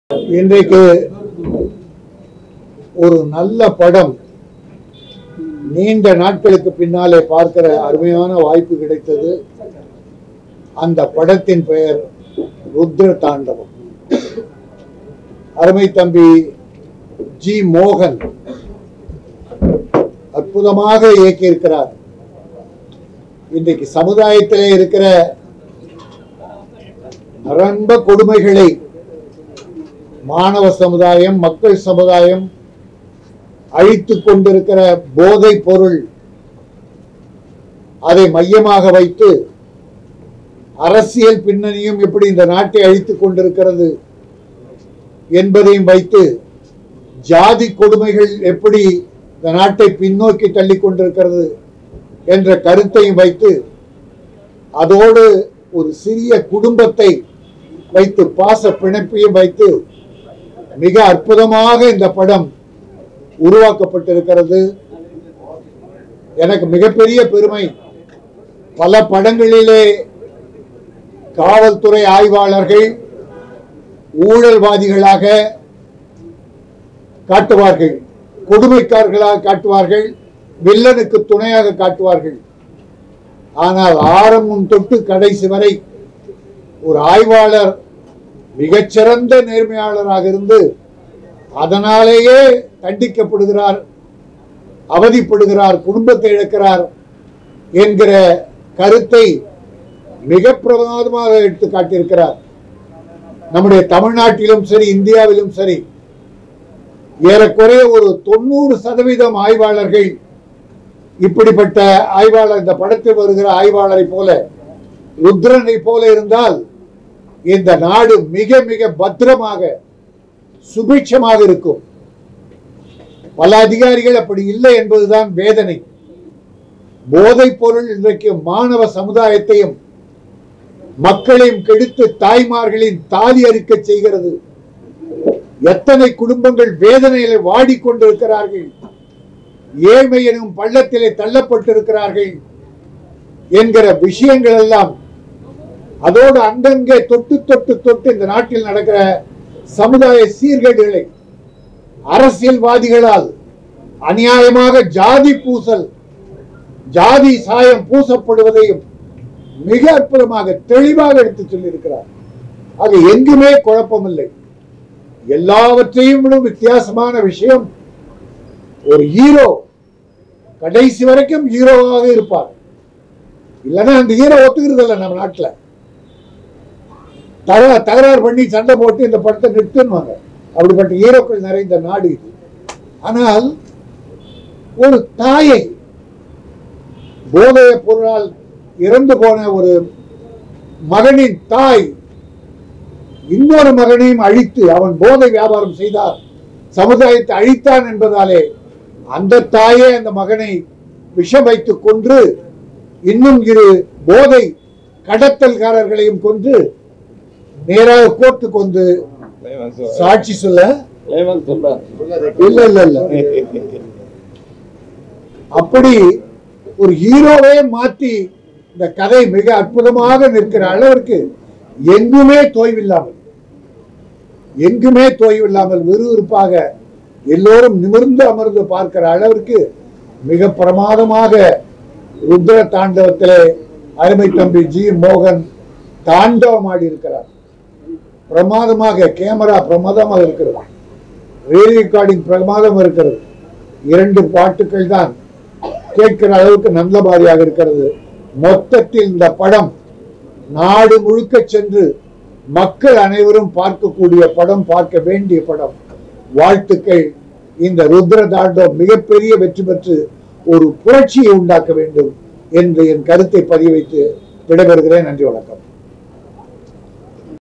மக்கள் அனைவரும் பார்க்க வேண்டிய படம் - k rajan Latest speech Rudra Thandavam Movie Premiere show
Rudra Thandavam படம் பார்த்த பிறகு - k rajan Latest speech Rudra Thandavam Movie